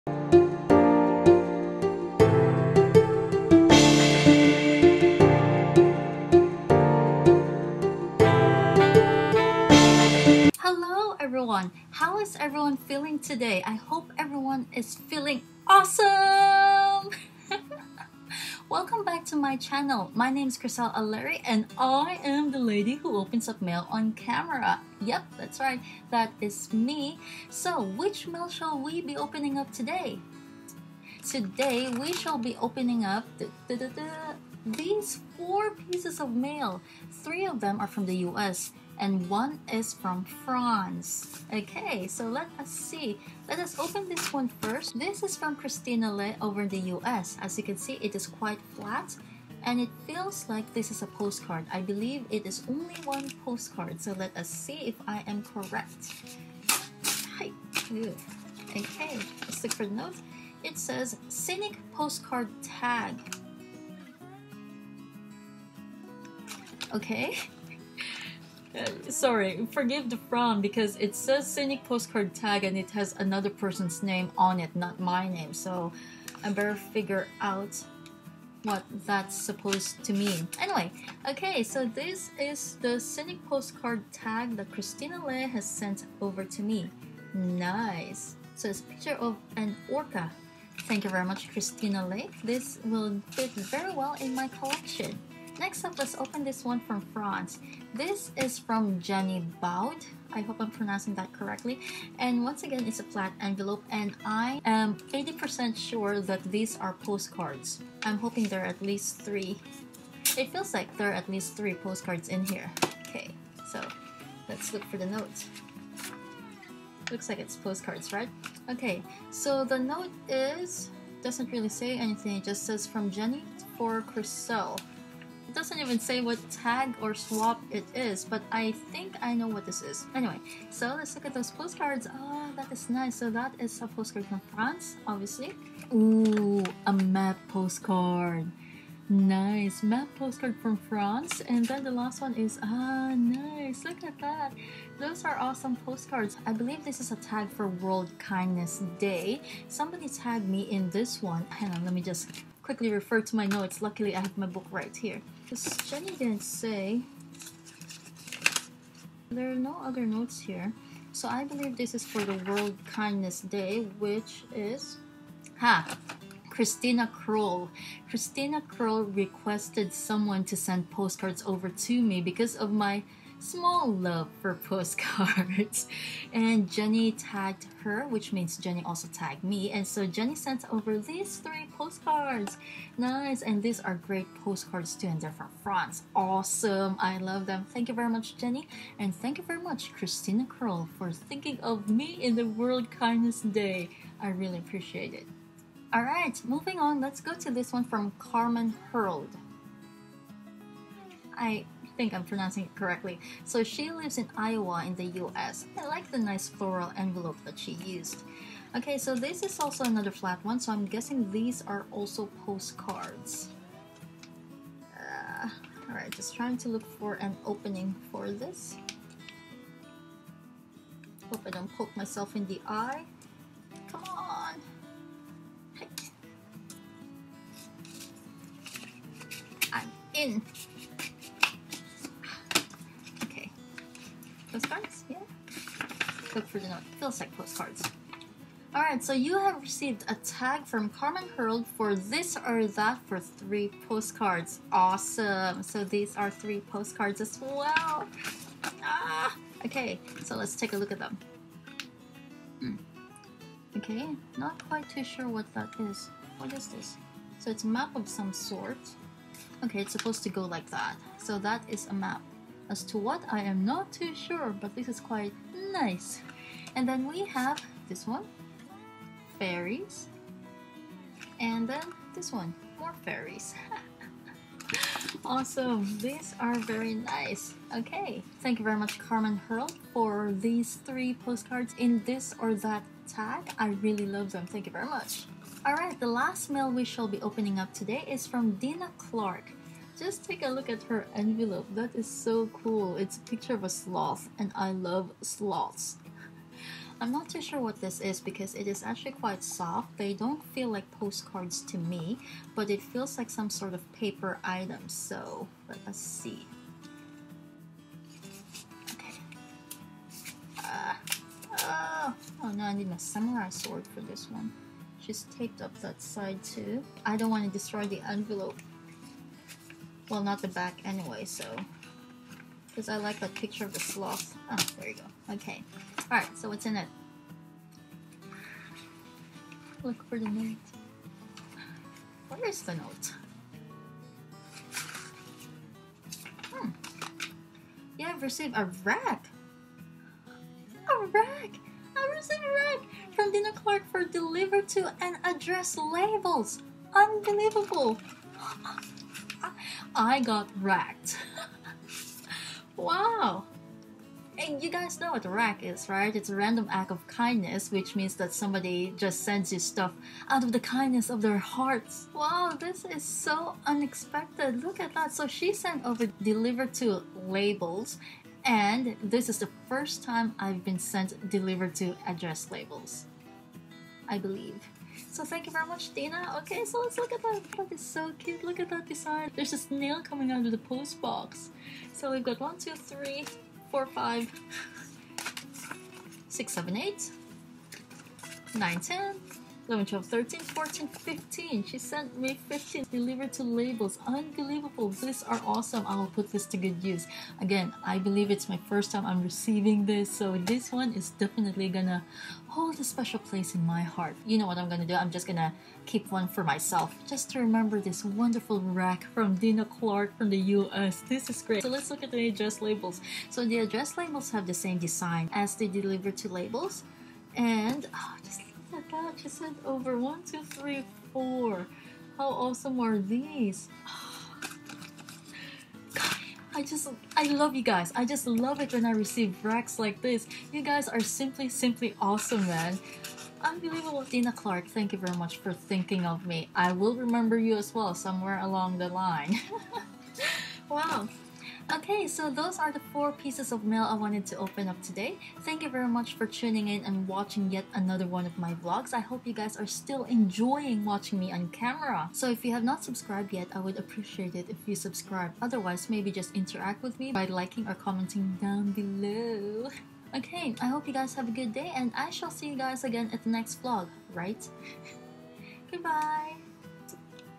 Incoming Mail | Postcards and a RAK 😀
This is a "flat items" vlog. A massive Thank You goes to the wonderful senders:
- Christina Le (USA)
- Jenny Baud (France) AND Christina Krol (USA)
- Carmen Heald (USA)
- Dina Clark (USA)